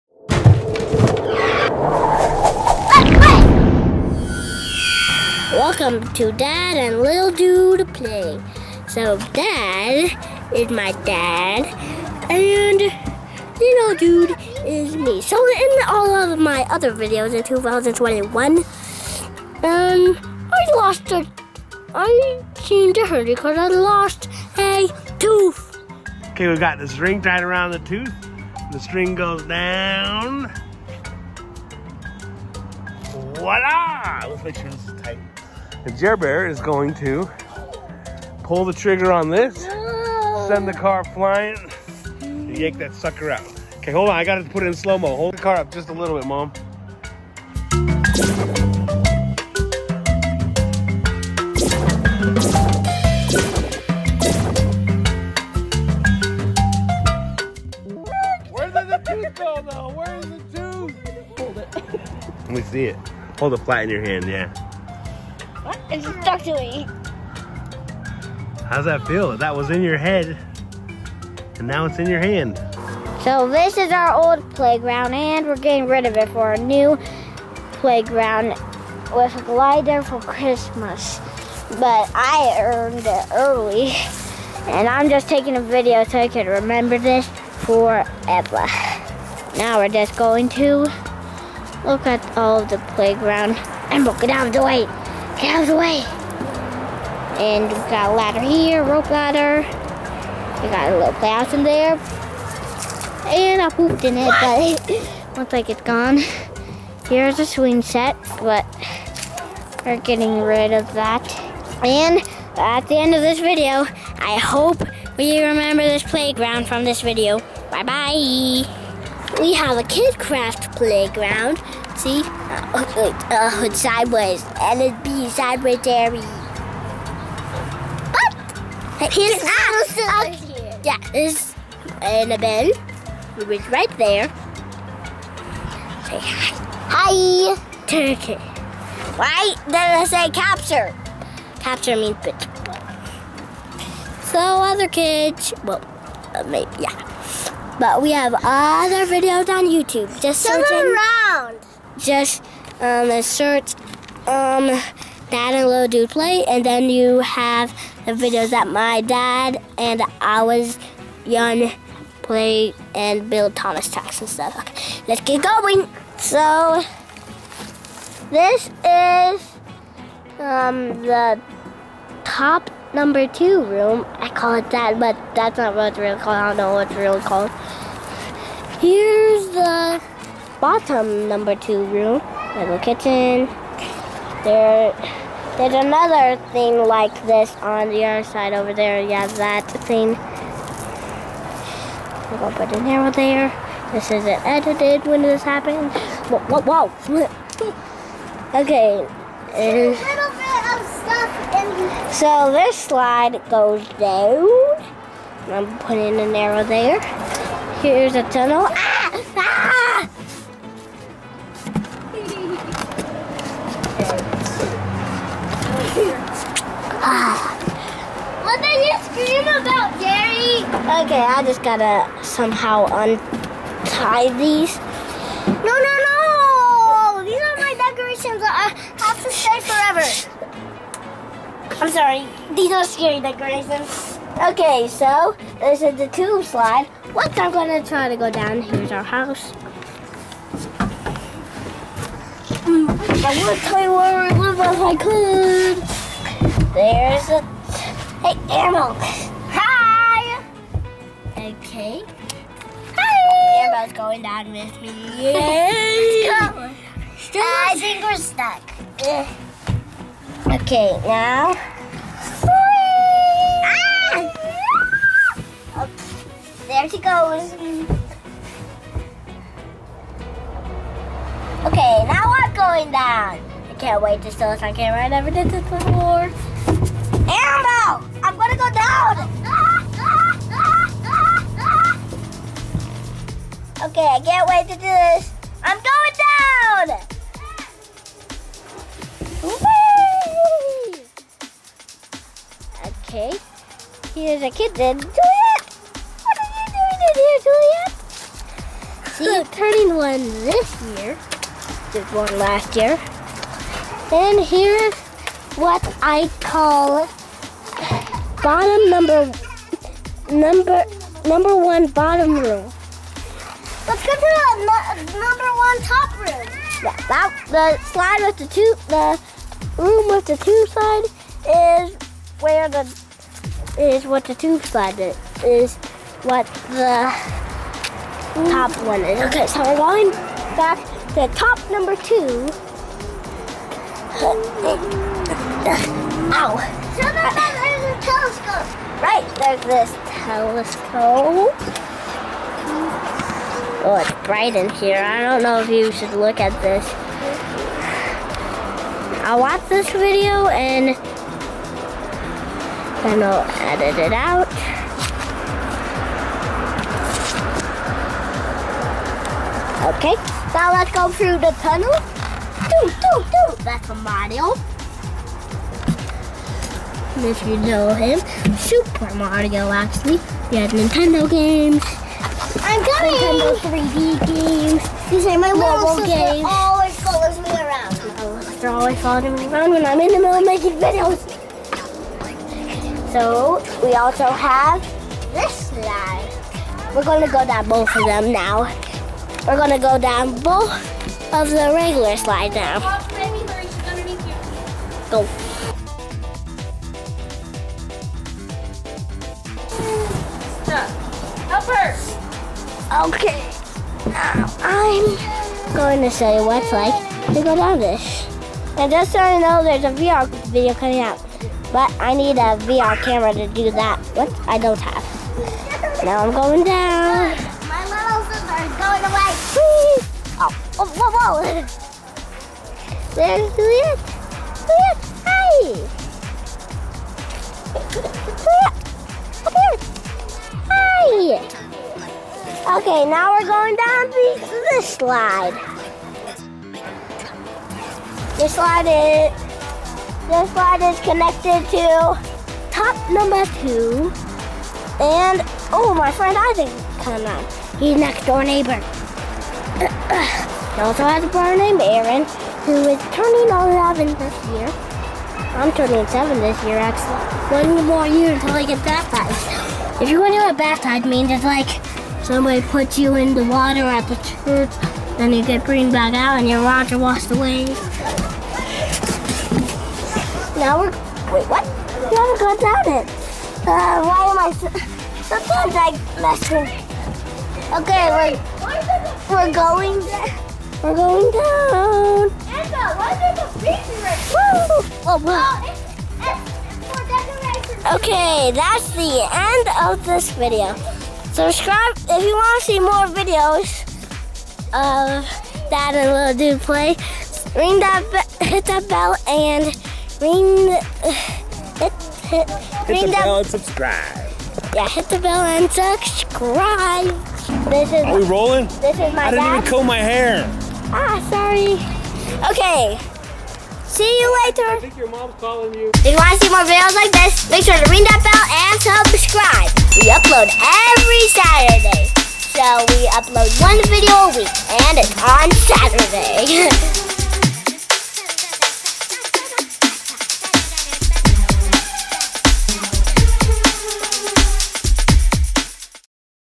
Tour of KidKraft playhouse, and Jerry loses a tooth!
In this video Jer Bear (aka Jerry) loses his first tooth with a little help from dad's remote control car. It was epic in slow motion to watch that tooth pop out of his head.

Also a tour of the KidKraft Paramount wooden playset. Dad and Mom put this thing together in about a week and it is AWESOME!